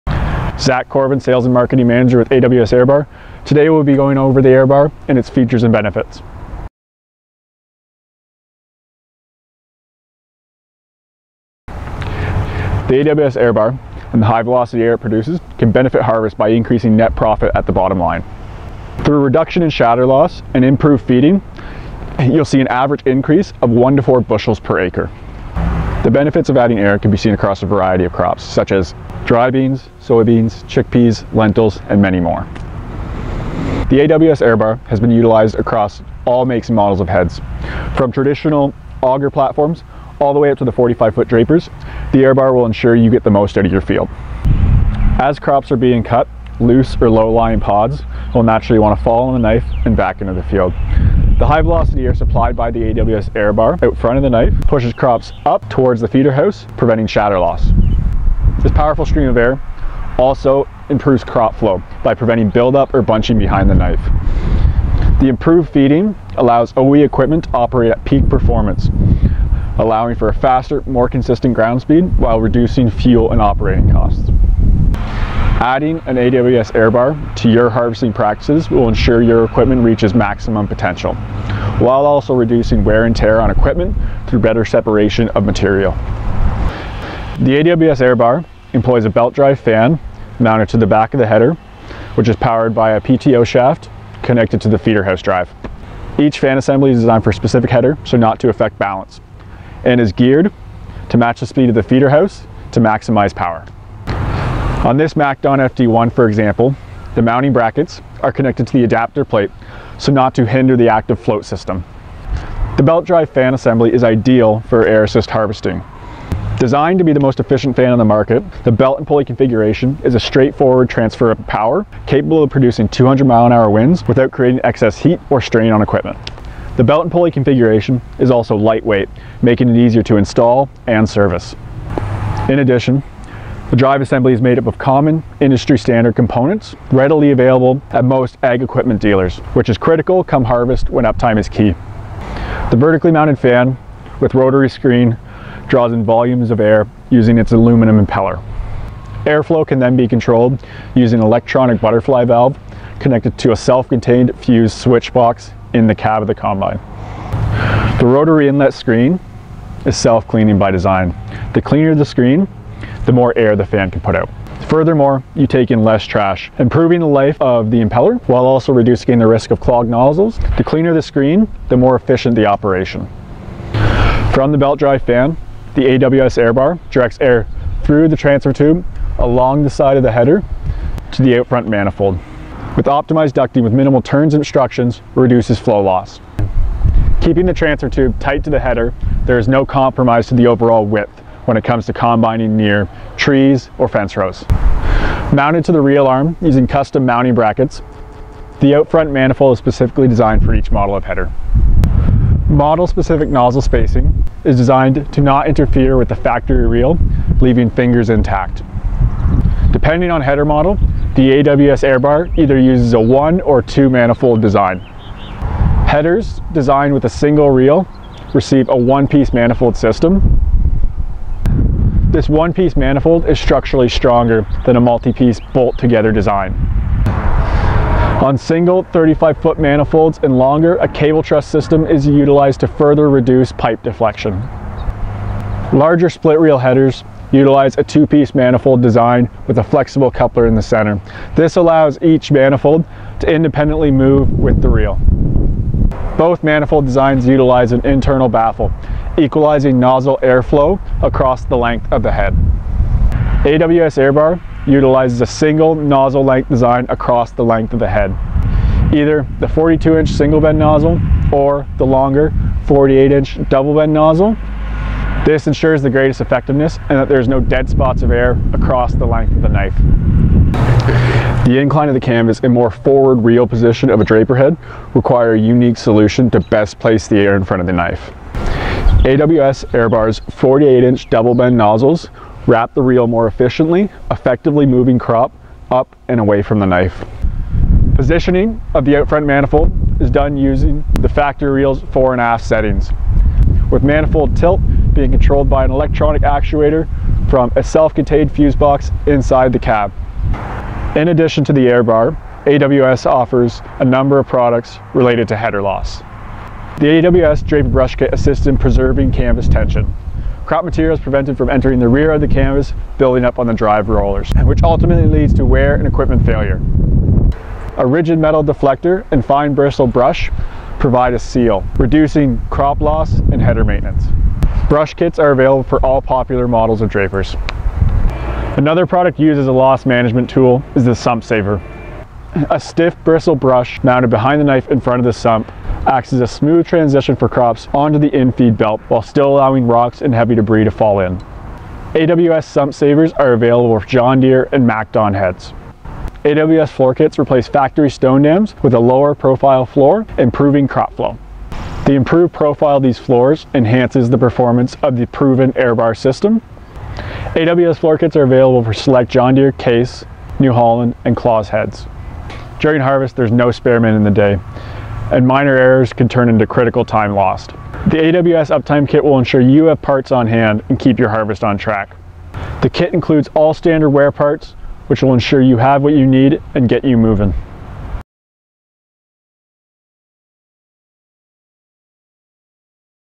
How an AWS Airbar can benefit your harvest.
The AWS Airbar and the stream of high velocity air it produces can benefit your harvest and increase net profit at the bottom line. Through reduced shatter loss and improved feeding capabilities you’ll see an average increase of 1-4 bushels per acre.

The benefits of harvesting with air can be witnessed across an array of crops such as Dry Beans, Soybean lentils, chickpeas and much more.

The AWS Airbar has been utilized on all makes and models of headers. From traditional auger platforms to 45’ drapers the Airbar will ensure you get the most out of your field.

As crops are cut, loss or low-lying pods will naturally land on the knife and fall back into the field. The high velocity air supplied by the Airbar to the front of the knife pushes crops back toward the feeder house as they are cut preventing this shatter loss. This powerful air stream also helps to improve crop flow, preventing build up and bunching. With the improved feeding capabilities OE equipment can operate at peak performance. Allowing for a fast more consistent speed, reducing fuel and operating costs.

Adding an Airbar to your harvest practices will not only ensure your maximizing your equipment’s potential it also reduces wear and tear on combine equipment through better separation of crop material.

AWS Airbar employs a belt drive fan assembly mounted on the back of the header, driven by a PTO shaft connected to the feeder house drive. Each fan assembly is designed to mount on specific headers so not to effect header balance and is geared to match the speed of the OE feeder house to deliver maximum power. On this Mac Don FD 135, mounting brackets are fix to the adapter plate so not to affect the active float system. AWS manufactures systems for all major header models.

The belt drive fan is the ideal power source for air assist harvesting. Designed to be the most efficient fan on the market, the belt and pulley configuration supplies a straightforward way of transferring power capable of producing 200 mph winds without creating excessive heat and strain on equipment. Without the need of a slip clutch it won’t add any excess strain to the feeder house drive while in operation. The belt and pulley design is lightweight, easy to install and service.

In addition, the drive assembly is made up of common industry standard components, making replacement parts readily available at most Ag equipment dealers. A key factor during a busy harvest when up-time is critical.

The vertically mounted fan with rotary screen inlet draws in volumes of air using a aluminum impeller. Air flow can then be controlled using a butterfly actuator valve controlled by self contained fused switch box located inside the cab.

Out-Front Manifold:
From the belt drive fan, the AWS Airbar directs air through the transition tube to the out-front manifold along the side of the header. The optimize ducting, with minimal turns and obstructions reduces flow loss.

Keeping the transition tube tight to the header, there is no comprising effects to the width of the header that could cause issues near fence rows or trees.

Mounted to the reel arms using custom mounting brackets, the aluminum manifold is designed to fit specific header models. Nozzle spacing is arranged to not interfere with factory reel, leaving fingers intact

Depending on the header model AWS utilizes either 1- or 2-piece manifold design. Headers with a single reel configuration receive a 1-piece manifold.
This 1-piece design is structurally stronger compared to a multi section bolt together system. On 35" + manifolds a cable truss system further minimizes deflection in the pipe.

On larger headers with a split reel design a 2-piece manifold is used with a flexible coupler joining the 2 manifolds together while still allowing each to move independently with the reels. On both manifolds styles incorporate an internal baffle, equalizing nozzle airflow across the full length of the manifold.

AWS Airbar utilizes a uniform nozzle length design. Using Single bend 42” or Double bend 48” depending on the specific header. This ensures the greatest effectiveness and that there are no dead spots of air across the knife.

The incline of the canvas and the more forward position of the reel of a draper head require a unique solution to best place the air stream. AWS Airbar’s 48” long double bend nozzles wrap the reel more effectively, and move the crop more efficiently from the knife.

Positioning of the out-front manifold are controlled by the factory reels fore and aft settings with manifold tilt being controlled by a single electronic actuator from a self-contained fused switch box located inside the cab.